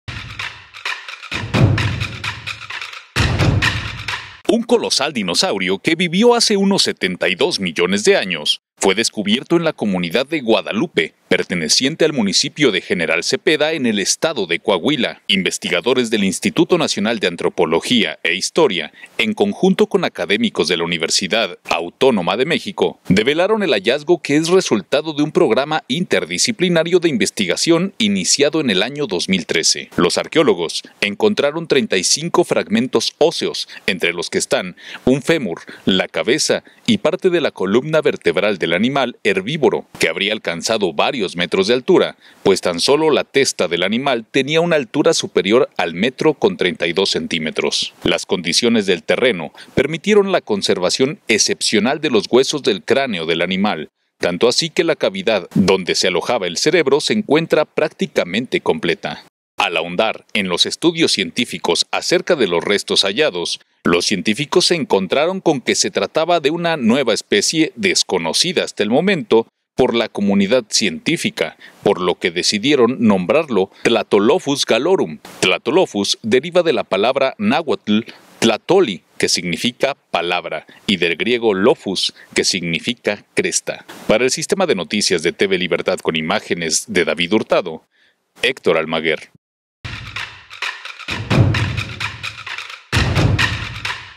Investigadores mexicanos descubren una nueva especie de dinosaurio en Coahuila.
Investigadores mexicanos descubren una nueva especie de dinosaurio en Coahuila, el reptil herbívoro habría vivido hace mas de 70 millones de años y su nombre deriva del náhuatl. 🦖 🕵🏼‍♂️